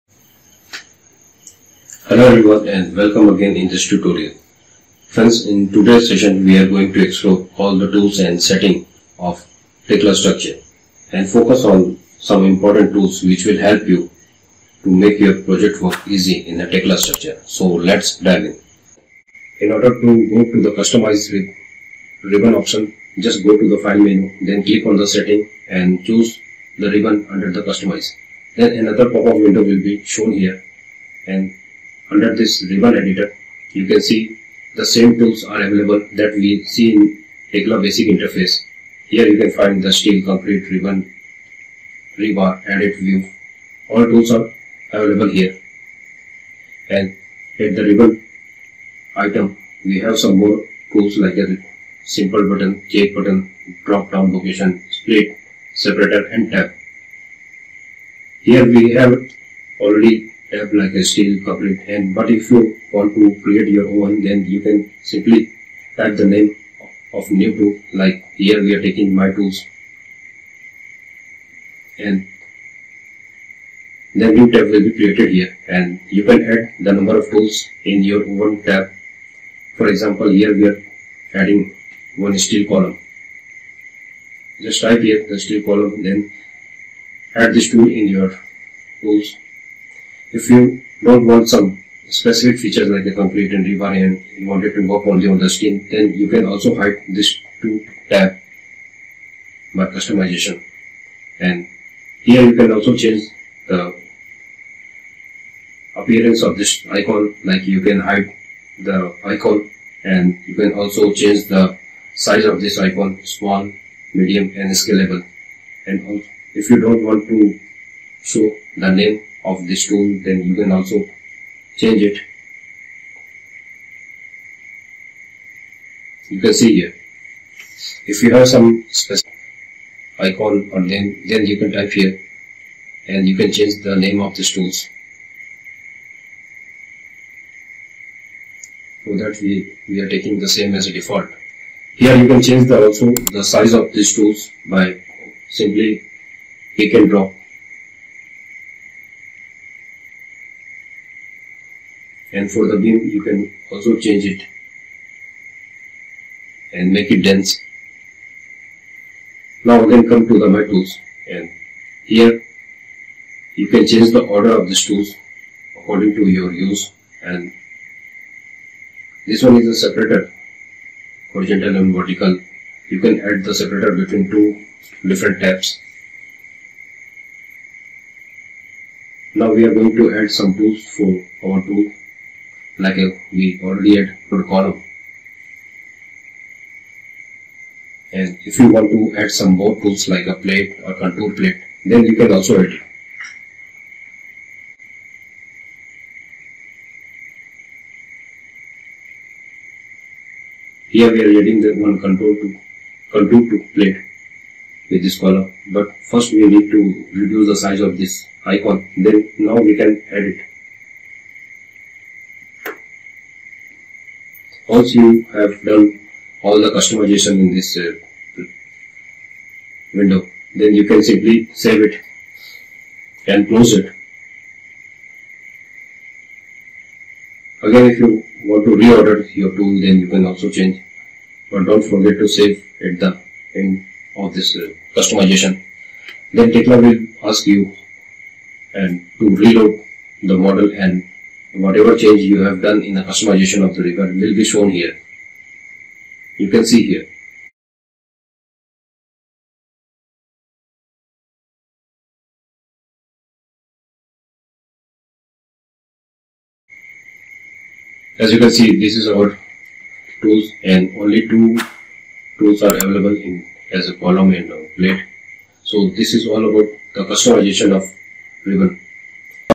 "CUSTOMIZE Tekla Structure 2025 Ribbon Tabs LIKE A PRO"!
Welcome to this Tekla Structures 2025 tutorial!

In this video, we explore how to customize the ribbon tab in Tekla’s user interface to match your workflow and boost productivity. Learn how to add, remove, or rearrange tools, create your own tabs, and save time with a personalized setup.

✅ What You’ll Learn:

How to access ribbon customization settings

Adding new tools to the ribbon

Creating custom tabs and groups

Best practices for UI layout personalization

Whether you're a beginner or experienced user, this tutorial will help you work smarter in Tekla Structures 2025.